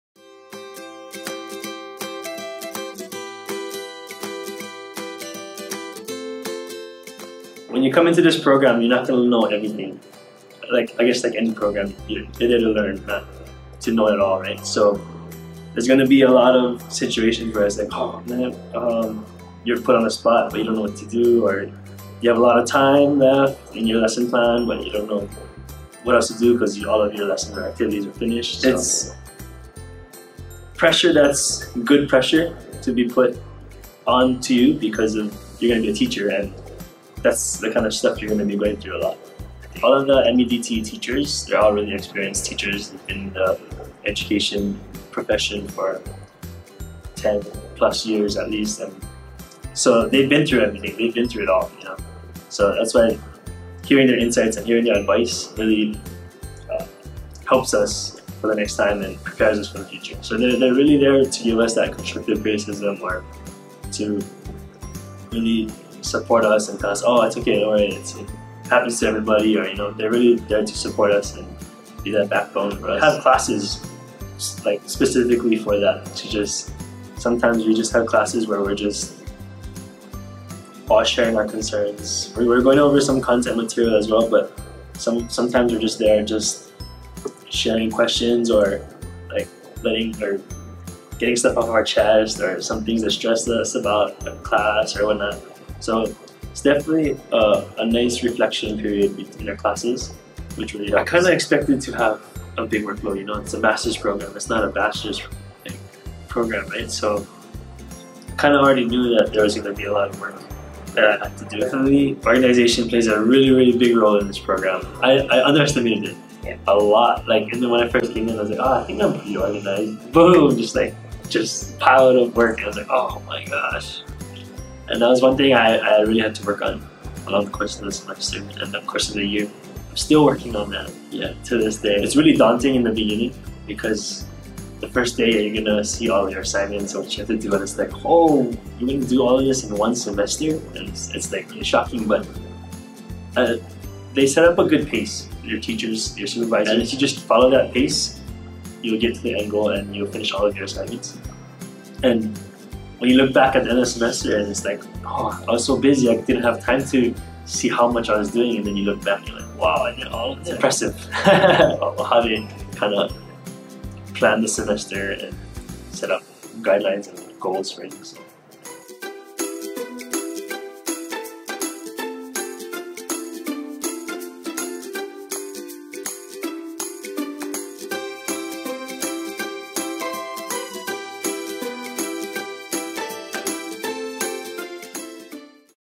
Jay Nakamura on Dealing with Stress
Jay Nakamura of Cohort 45 talks about dealing with stress while in the program.

Do you have a love for math and want to get licensed? Consider the University of Hawai'i at Manoa's Master of Education in Teaching Program!